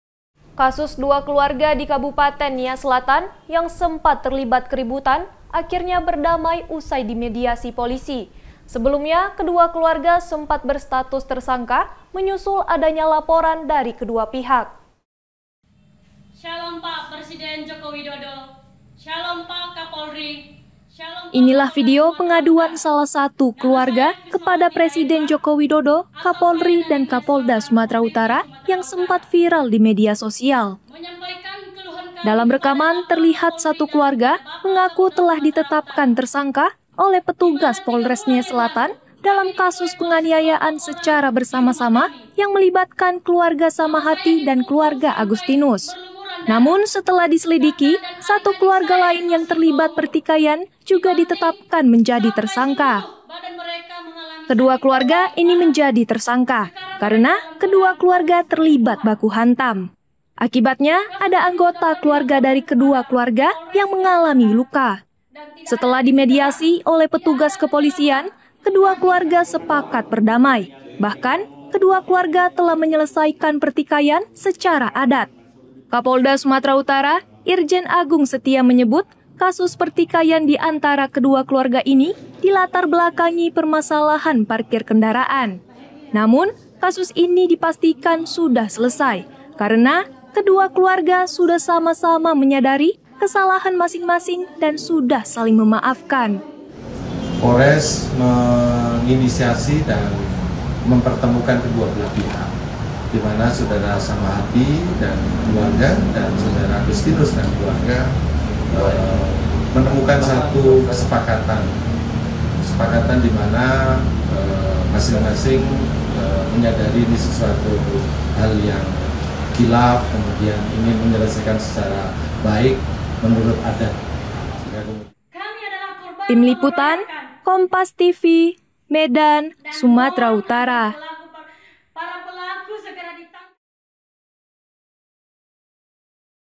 Pertikaian 2 Keluarga di Nias Selatan Berujung Damai
MEDAN, KOMPAS.TV - Kasus 2 keluarga di Kabupaten Nias Selatan yang sempat terlibat keributan akhirnya berdamai usai dimediasi polisi.

Sebelumnya, kedua keluarga sempat berstatus tersangka menyusul adanya laporan dari kedua pihak. (*)